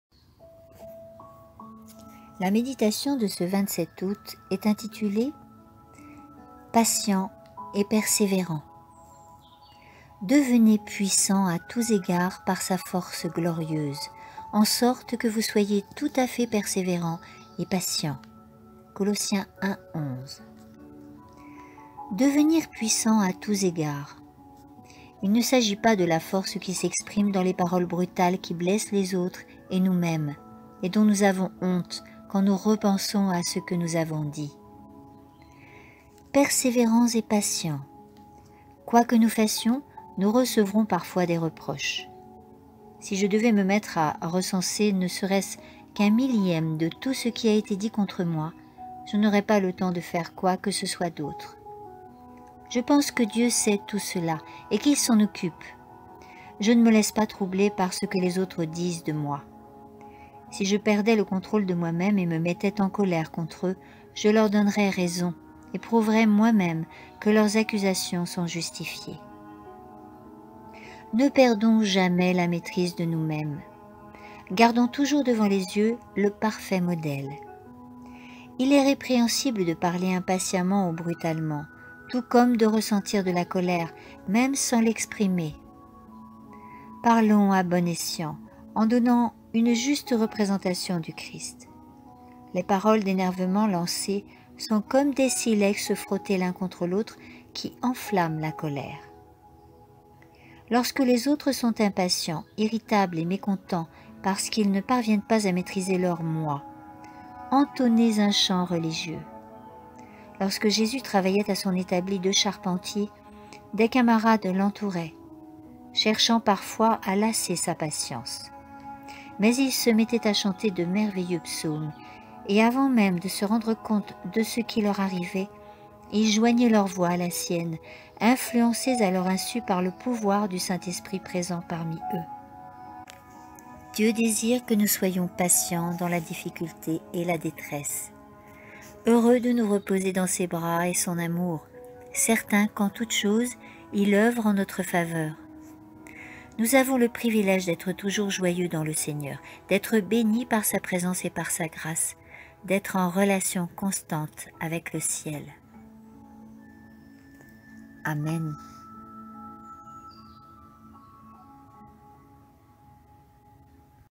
Dans les lieux Célestes - Méditation du 27 août de l'année en cours.
Méditation tirée du livre "Dans les lieux Célestes" d'E.G. WHITE.